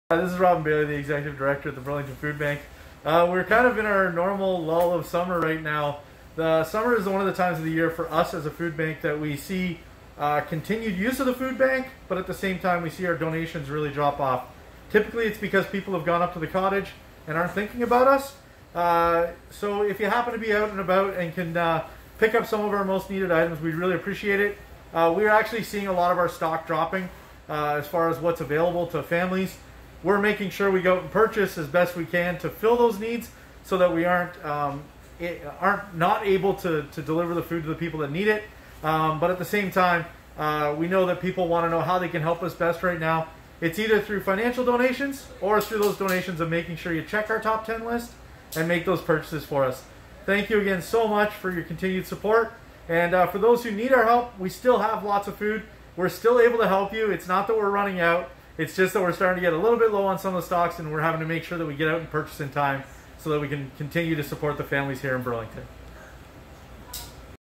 Burlington Food Bank update with Robin Bailey – July 27
Right now we are in the middle of our slowest season for the food bank. The deliveries keep going although right now we’ve seen a quick drop in orders. Could be the heat! Also during July and august we typically don’t receive any donations through the doors as events slow down and especially now with baseball and slo-pitch seasons being cancelled. It was great in previous years to see the kids teams rally behind the Gift of Giving Back and the Oldtimers Slo-pitch league and Dads Group running very successful tournaments, helping out families during the slowdown. We do make sure to purchase all of the top or main items we need so that we’re never short so all the monetary donations Burlington people have sent us are being put to good use now.